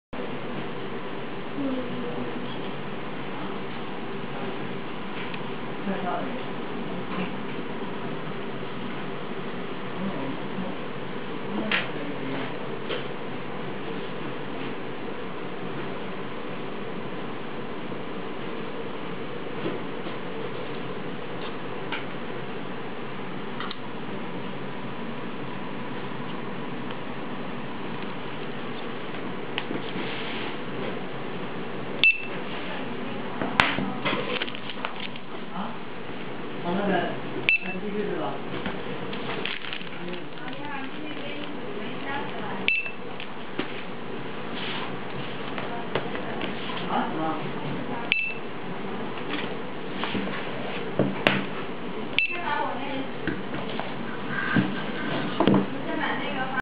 Use iPad barcode scanner! Make management easier!
Best friend of  retailors and wholesalers! See how a mobile barcode scanner work in book bussiness.

SUMLUNG SL-BA10 HID version released! It can support scan barcode to iPad, iPhone, iTouch, Windows Mobile, PC.

No need to install any driver, the barcode data will be output in active input window.

You can track shipping details online(manage ) using you iPad, iTouch or iPhone, isn't it great?